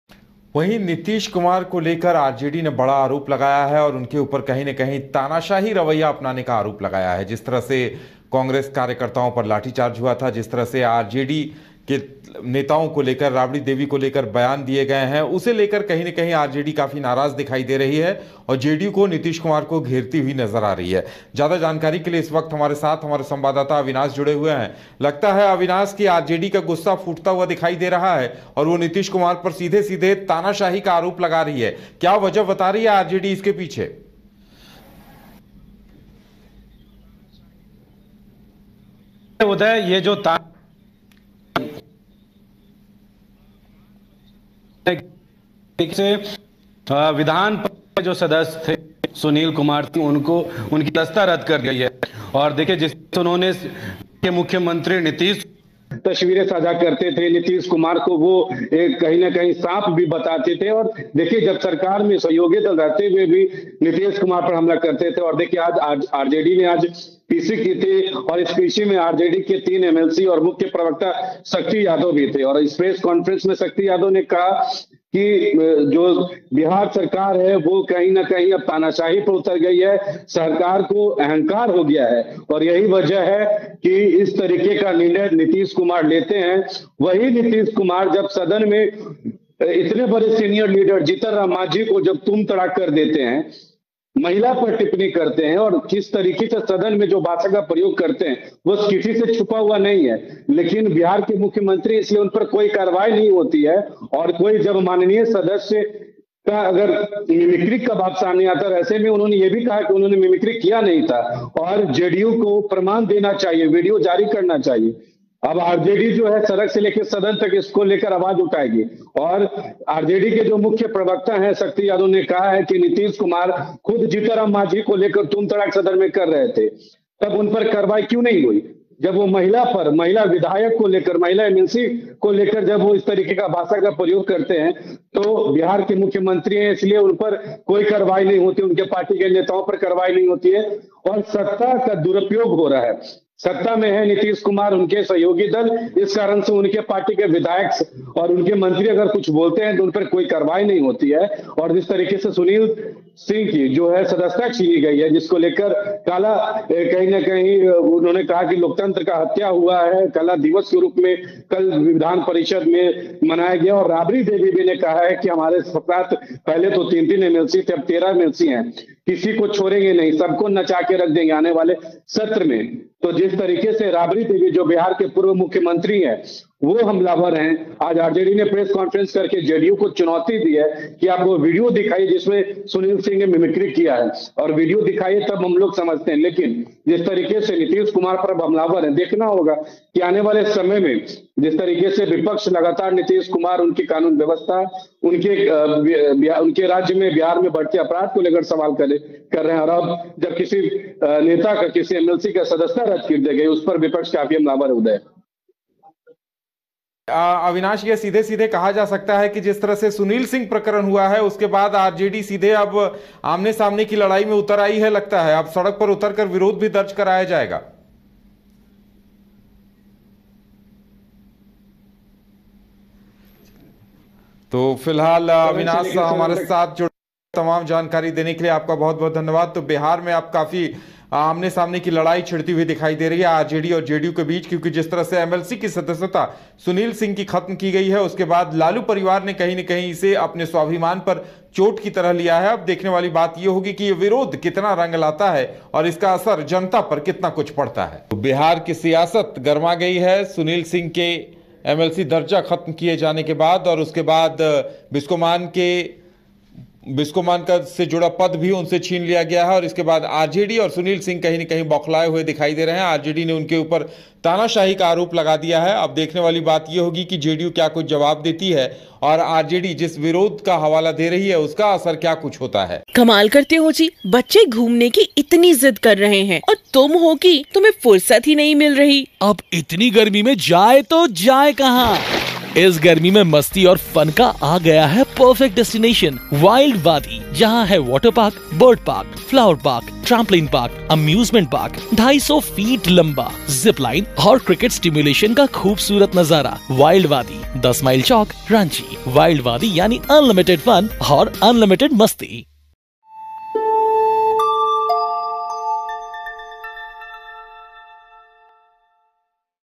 सुनील सिंह प्रकरण के बाद बौखलाए राजद का नीतीश के खिलाफ बड़ा एलान
RJD upset after Sunil Singh incident, big announcement against Nitish

22Scope Live TV | 22 Scope | Hindi News | 22 Scope लाइव

CHANNEL AVAILABLE
JIO TV Channel NO 1475
VI
ROCK TV